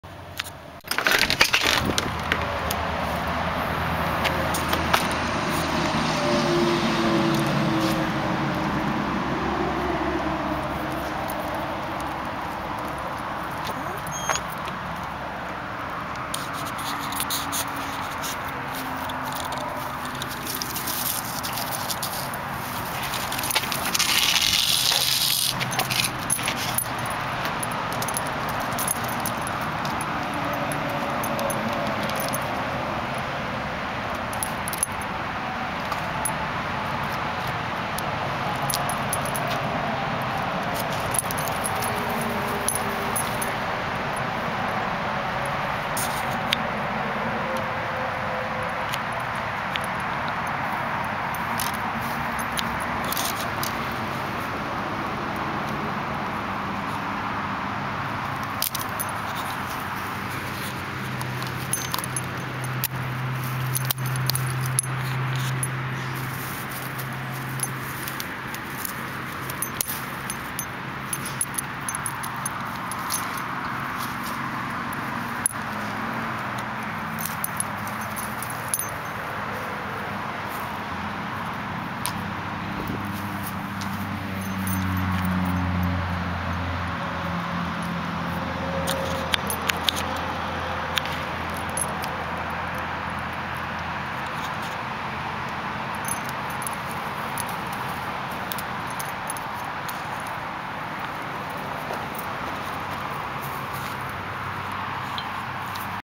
Dog playing with stick.
Astaire chews on a stick in the backyard next to I75.